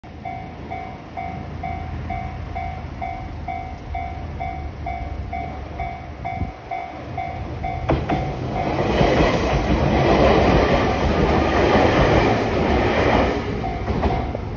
😍My Favorite Train crossingtrain pink train
japan train pink color my favorite color for train I love it this video my original upload viewing place toyohashi Minami Sakae Aichiken Japan @EliKatovlogs